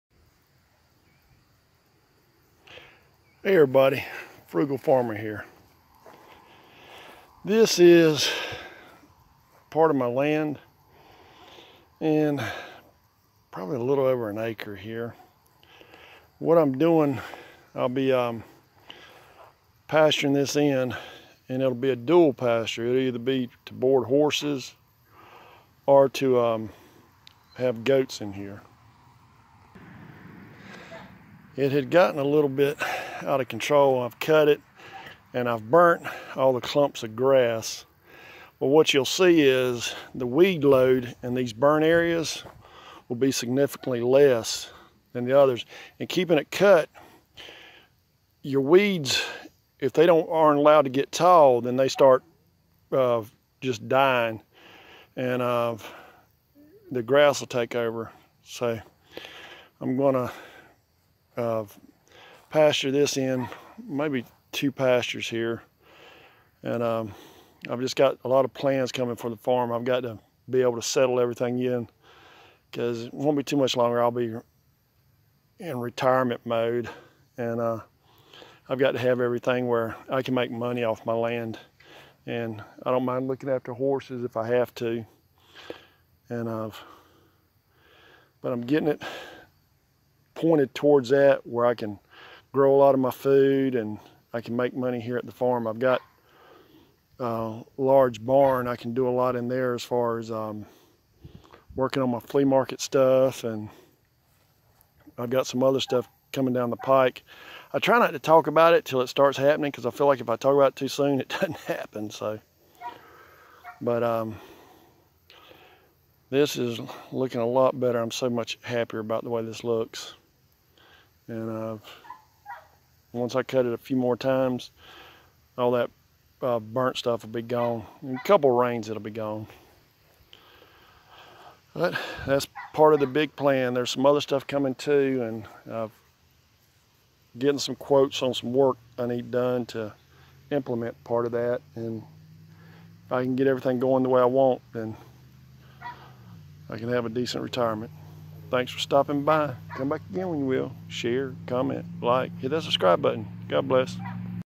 Longterm plans & changes coming to the farm.
I am looking more seriously about needed changes to my farm to make it a revenue stream for my retirement (which is getting closer at a very fast pace). I will be sharing these changes with you as I impliment them. Thank you for coming along for the ride.

Join our PayPal Friends Club and help us grow our channel, cat/dog rescue and farm.